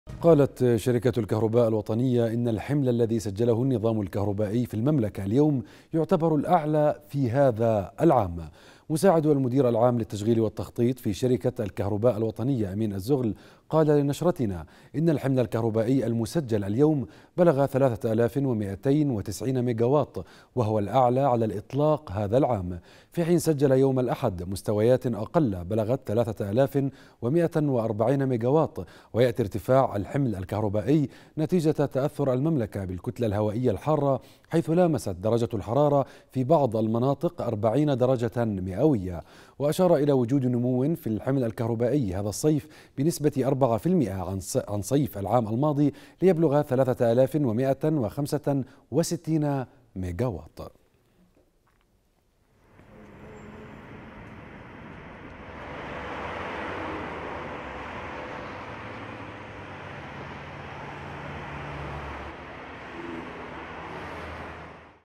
الحمل الكهربائي اليوم يسجل أعلى مستوى له في هذا العام - (17-7-2017)
قالت شركةُ الكهرباء الوطنية إن الحملَ الذي سجلهُ النظامُ الكهربائي في المملكةِ اليوم يعتبرُ الاعلى في هذا العام، وبلغ بلغَ ثلاثهَ الافٍ ومئتين وتسعينَ ميغاواط.